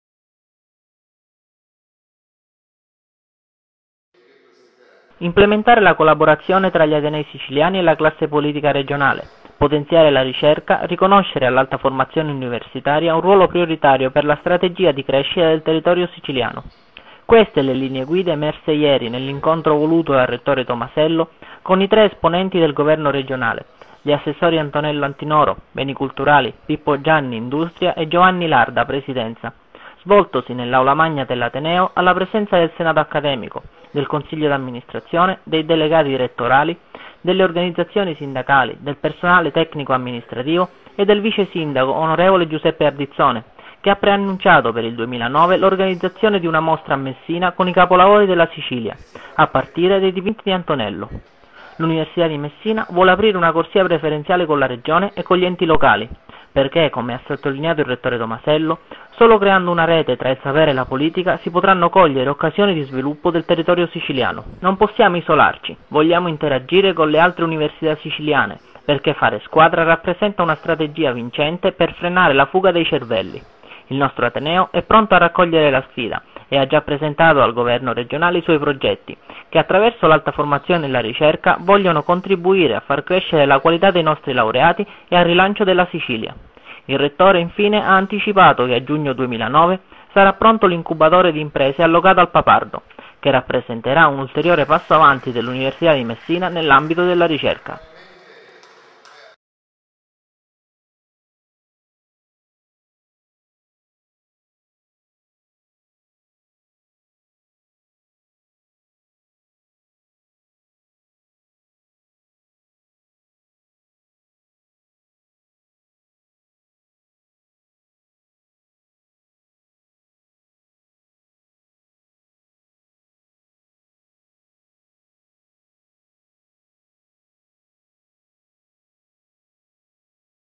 Sinergia Università-Regione per il rilancio della Sicilia
Incontro tra il rettore, prof. Francesco Tomasello e gli assessori della Regione Sicilia, Gianni, Ilarda e Antinoro che si è svolto nell'Aula Magna dell'Ateneo peloritano